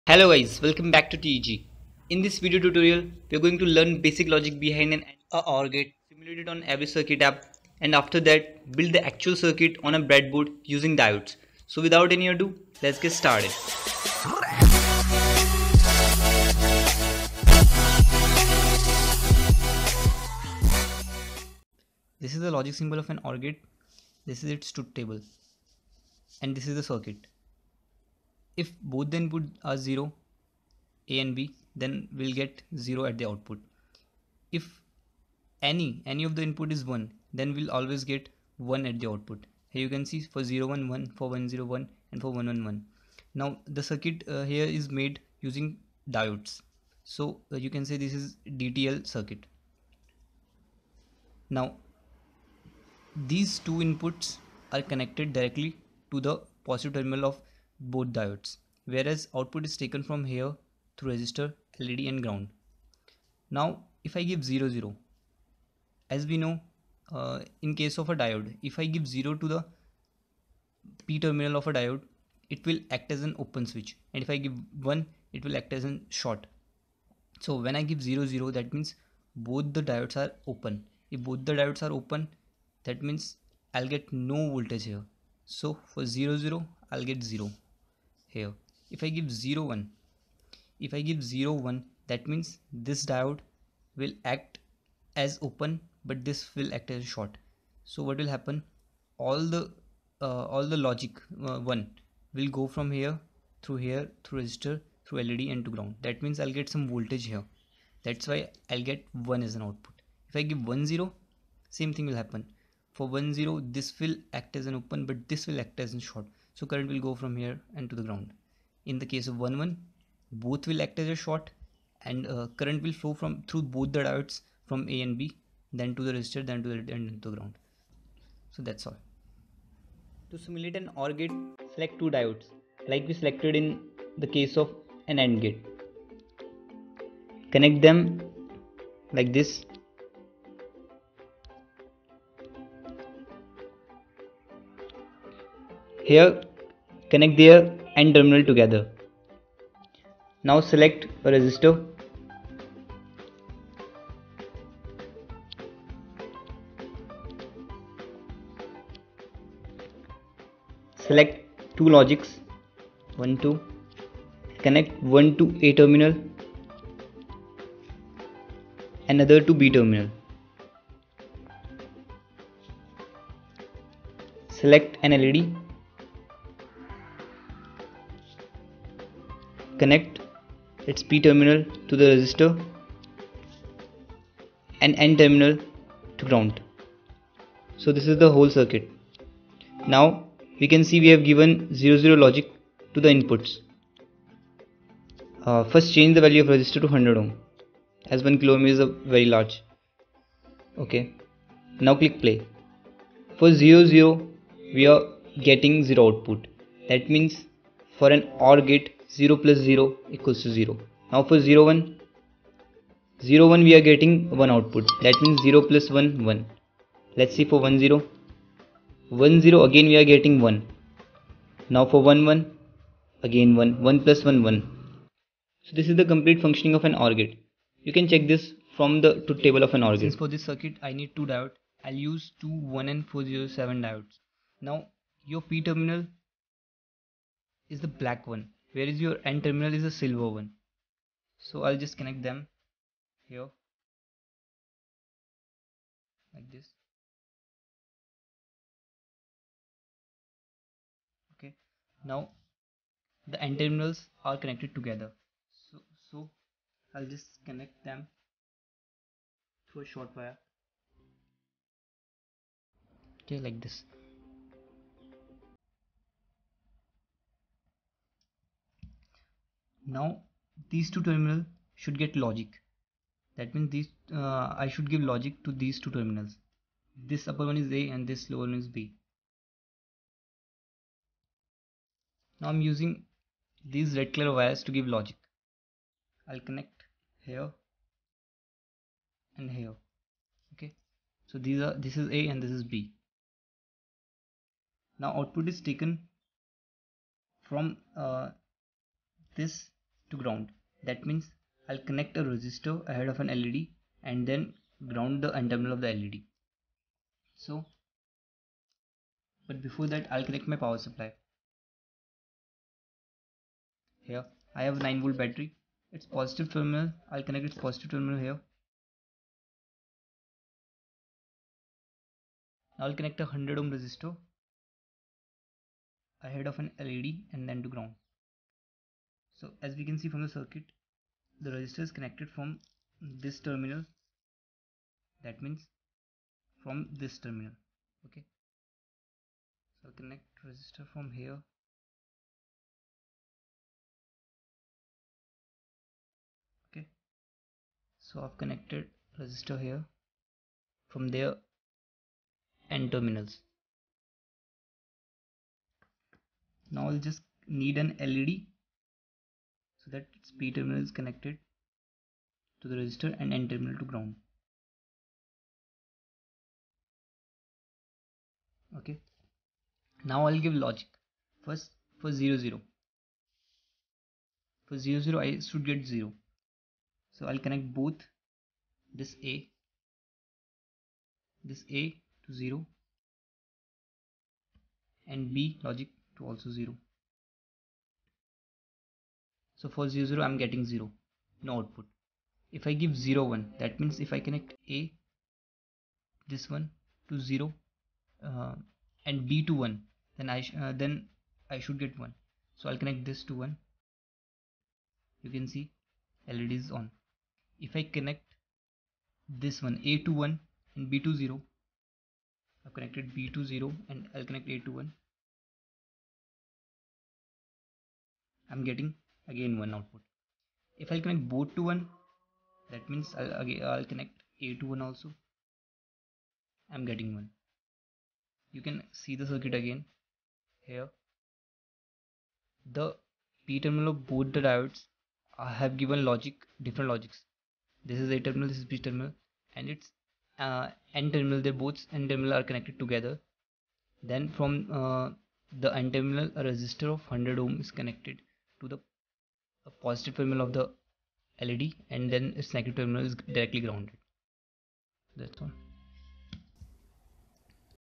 OR Gate Using Diodes (Everything Explained!)
Logic gates are the basic building blocks of any digital system. It is an electronic circuit having one or more than one input and only one output. The relationship between the input and the output is based on a certain logic. Based on this, logic gates are named as AND gate, OR gate, NOT gate etc

                                  OR GATE::
 A HIGH output (1) results if one or both the inputs to the gate are HIGH (1). If neither input is high, a LOW output (0) results. In another sense, the function of OR effectively finds the maximum between two binary digits, just as the complementary AND function finds the minimum.

SCHEMETIC-- In this section of video you will learn Logic symbol, Truth table of an OR gate. Circuit of OR gate using DIODE TRANSISTOR LOGIC is given.

SIMULATION --In this section simulation of the OR Gate circuit is done using DTL. App used for simulation is "EVERY CIRCUIT"  link is given below:

Working of the circuit in accordance with the truth table is also explained using simulation.

MAKING THE CIRCUIT-- For building OR gate circuit components used are:
1. BREADBOARD for connection.
2.1k resistor
3.100 ohm resistor.
4. 1N4007 DIODE(2 pieces).
5.LED
6. 9 VOLT BATTERY